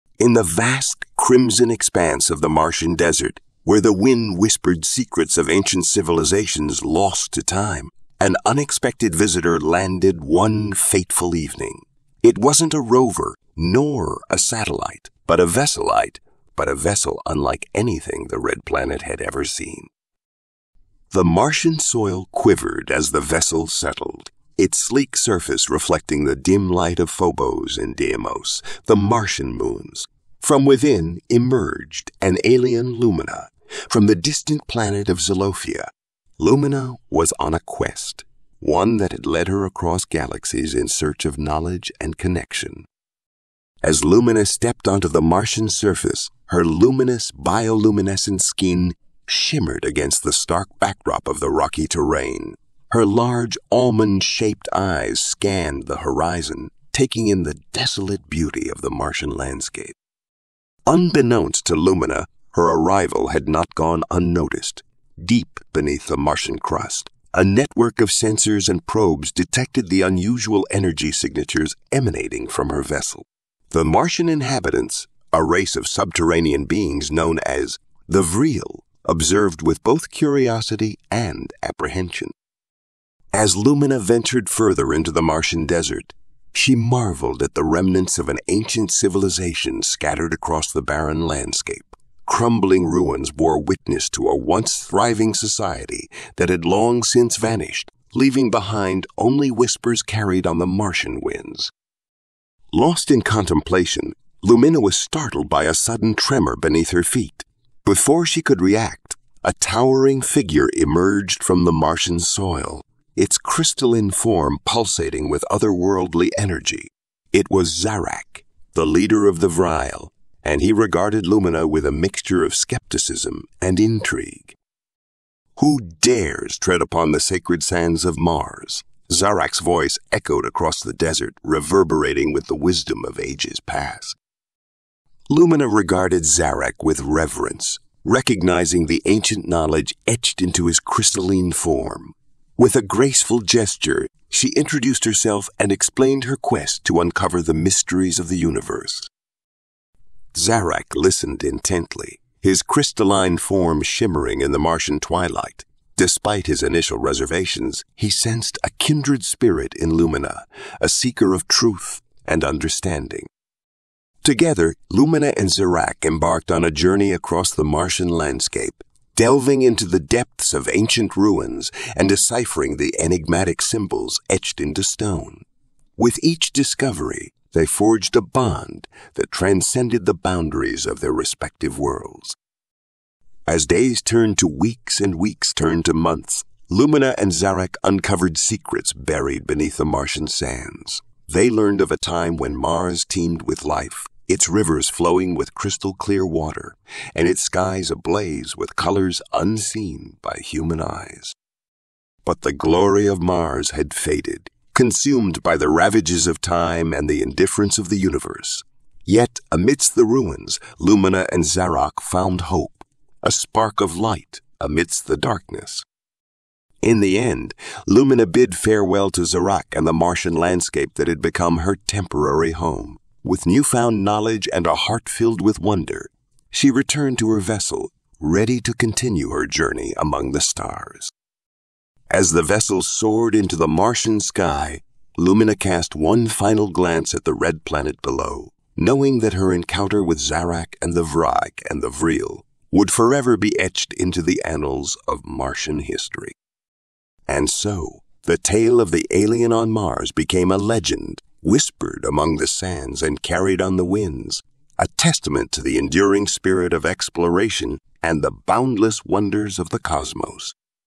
Alien Encounter on Mars: Unraveling Mysteries of the Red Planet
Join us on an extraordinary journey as an alien visitor, Lumina, from the distant planet of Xylophia, explores the ancient ruins and encounters the enigmatic Vril beneath the crimson skies of Mars. Follow along as Lumina and Zarak delve into the secrets buried within the Martian sands, unraveling the mysteries of a world lost to time. Witness a tale of friendship, discovery, and the enduring spirit of exploration amidst the barren expanse of the red planet.